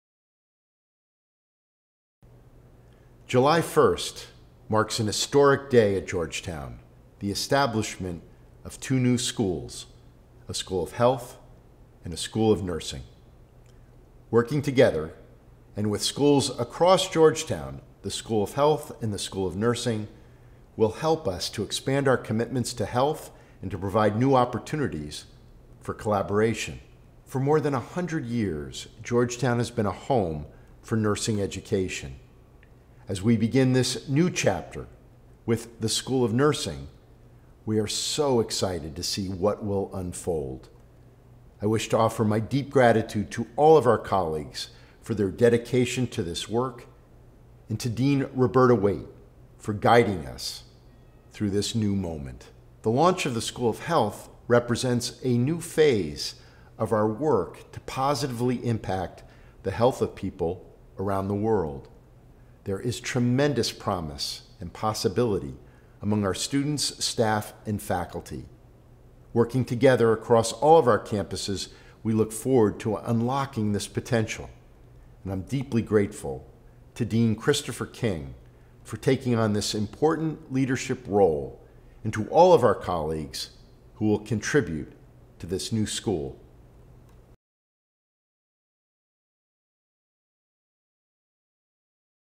Georgetown Launches School of Nursing and School of Health
On July 1, 2022, @georgetownuniversity President John J. DeGioia shared an important message about the historic launch of the School of Nursing and School of Health.